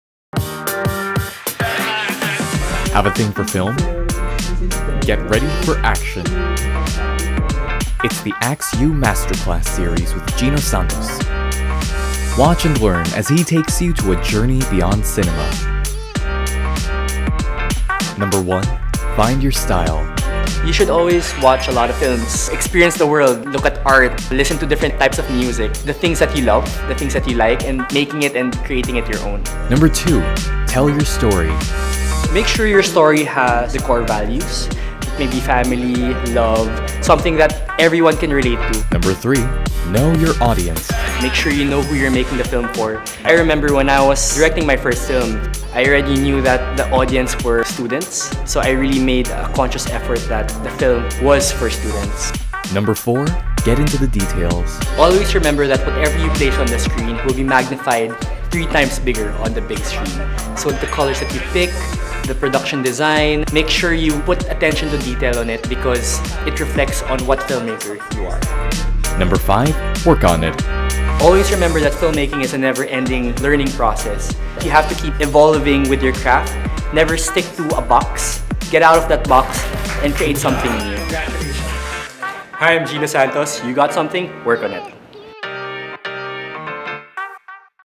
Have a thing for film?
Missed our Beyond Cinema masterclass? Get ready for action with tips from Gino Santos.